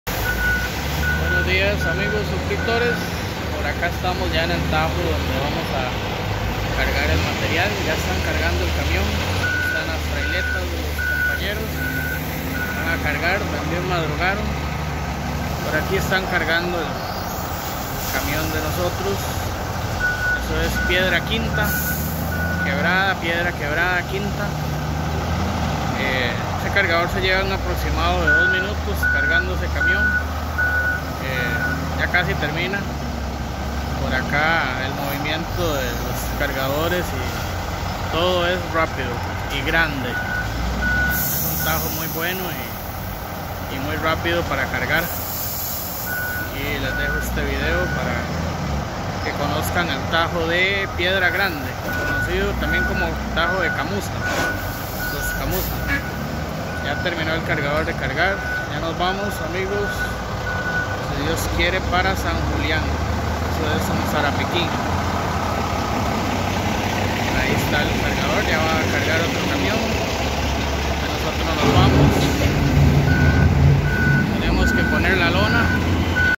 "Un Tajo Increible 🇨🇷 Tajo Piedra Grande "
Buenos días cargando en un tajo en Río Blanco Costa Rica Tajo Piedra grande 🚜🚛🚛🚛🚛🚛🚛🚛🇨🇷